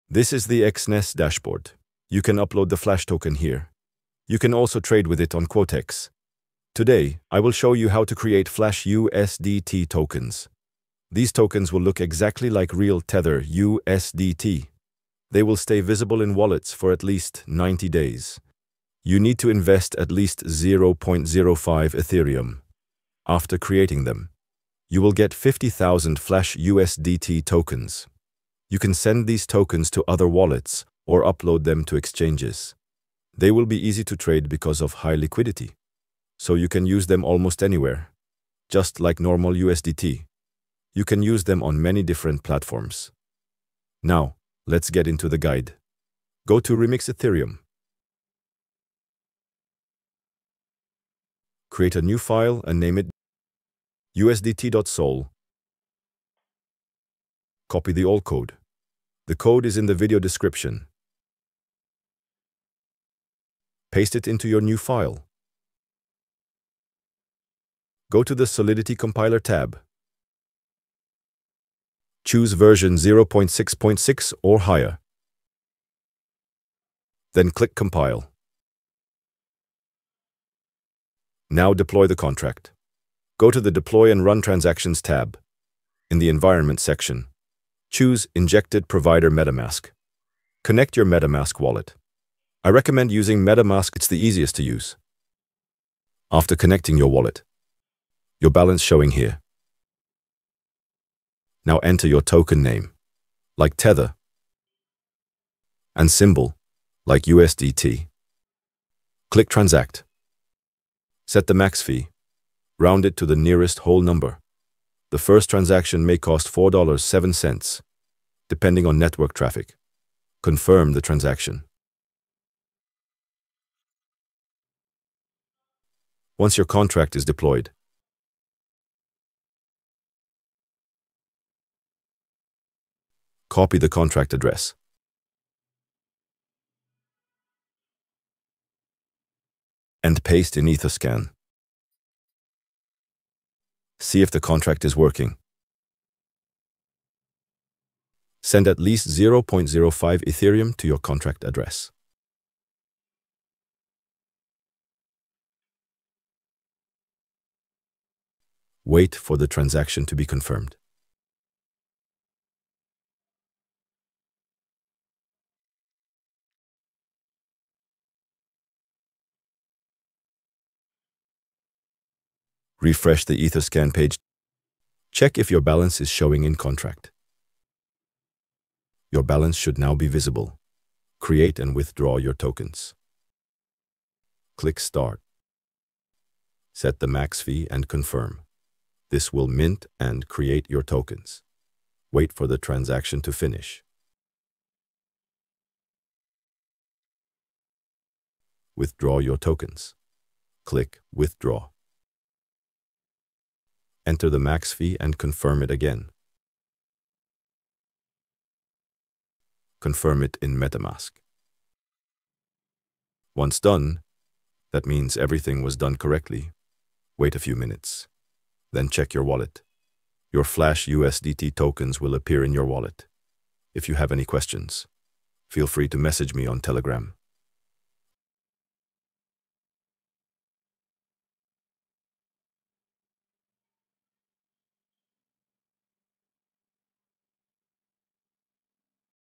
Create Flash USDT on Ethereum Network | Easy ERC20 & TRC20 Tutorial
🔓 Code ✅

🔓 Remix ✅

remix.ethereum

Welcome to this easy guide on how to create Flash USDT! 🚀

In this video, we’ll show you step by step how to make Flash USDT — a special kind of digital money that you can trade and send to others.

It doesn’t matter if you’re new to crypto or already know a lot — this tutorial will help you understand how Flash USDT works and how to use it the right way.

🔹 What You’ll Learn:

What Flash USDT is
How to make Flash USDT step by step
Tips to keep your Flash USDT safe
How to trade and send Flash USDT to others